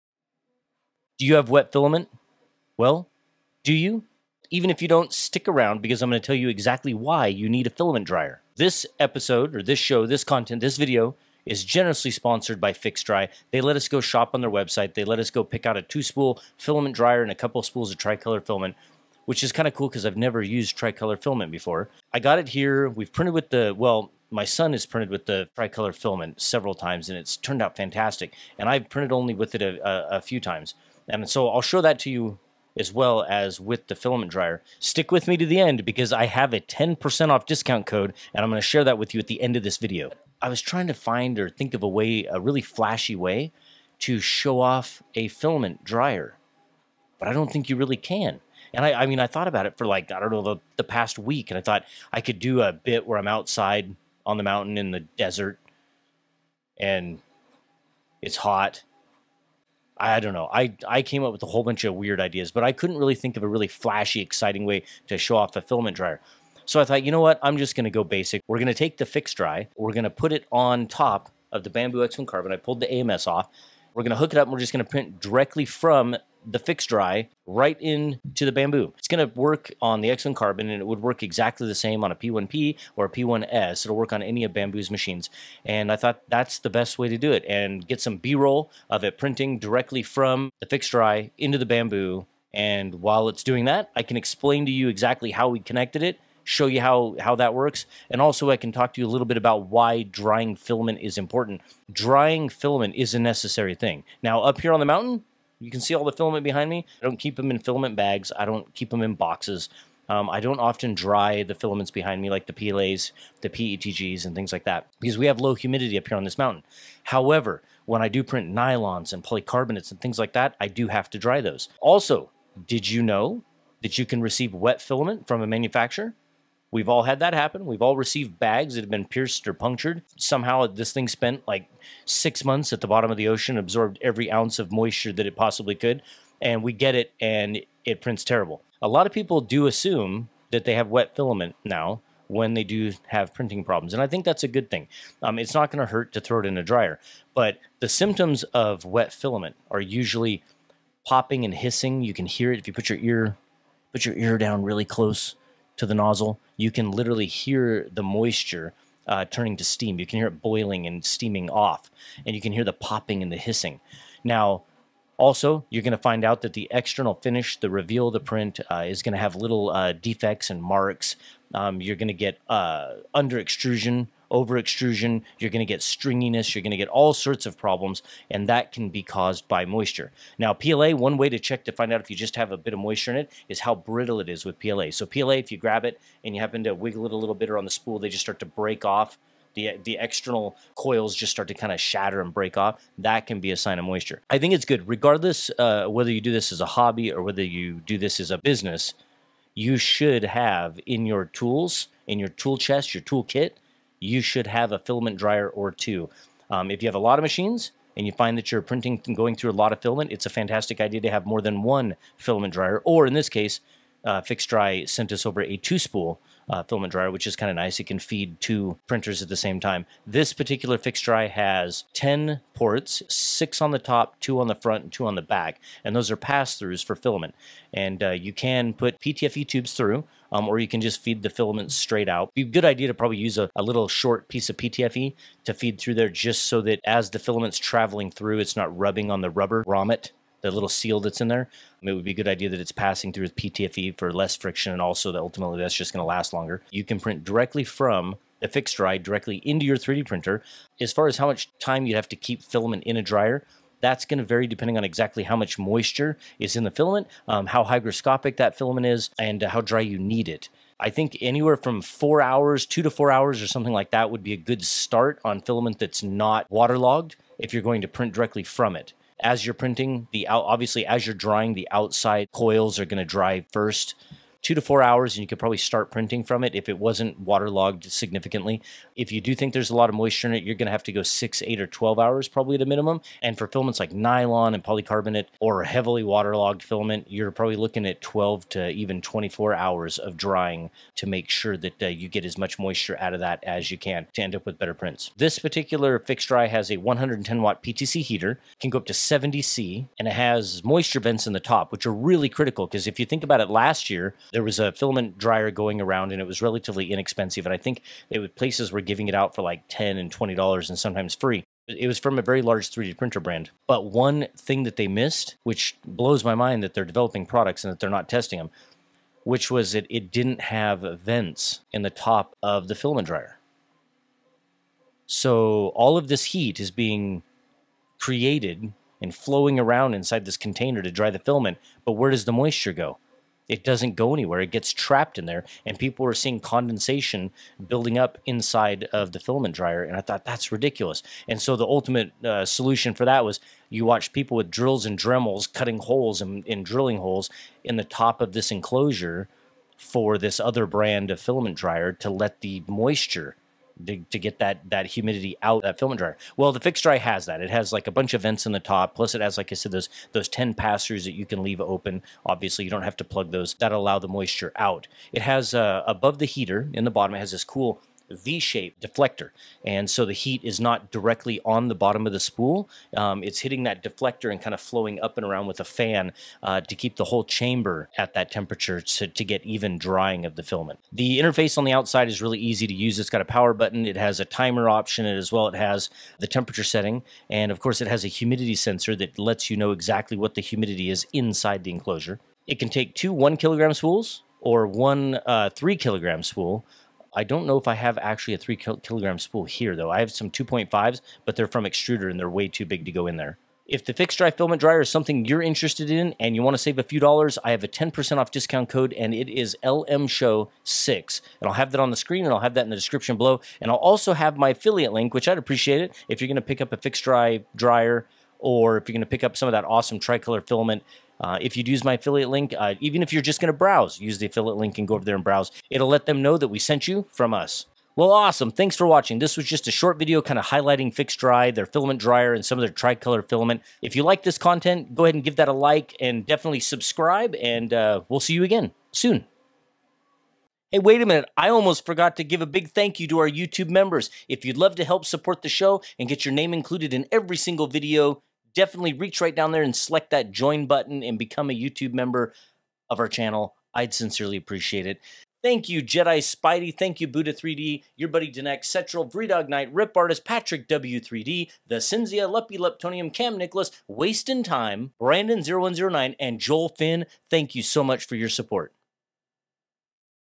NEVER AGAIN! FixDry Filament Dryer on the Bambu X1C, P1P & P1S
If you have water logged filament or a high humidity and moisture, then you should DEFINITELY have a filament dryer! Actually, even if you don't I am going to explain why you should have one anyway!

I really tried to come up with a flashy way of showing off a filament dryer, but in the end, I think the best way was to simply connect the FixDry filament dryer to a 3D printer, in this case a Bambu Lab X1C, and just print with it! Connecting it to the X1C is exactly the same with the P1P and P1S, and of course you can use a filament dryer with ANY 3D printer.